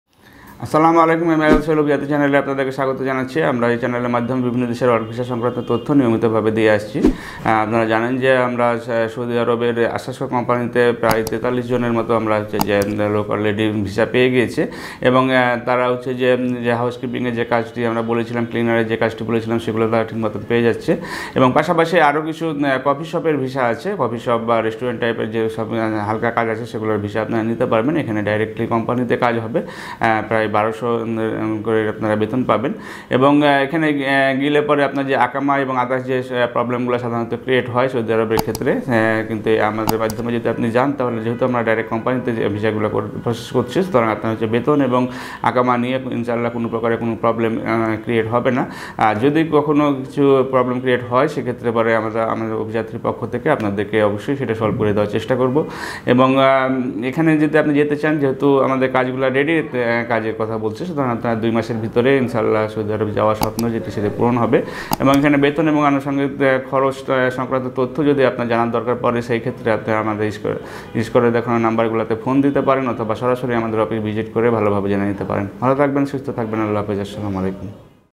সৌদি আরবে কোন কাজের বেতন বেশি? সৌদি ভিসা প্রসেসিং | Saudi Arabia Visa Update 2024 | Company Job
1. Student Visa Process
2. Work Visa Process
3. Tourist Visa Process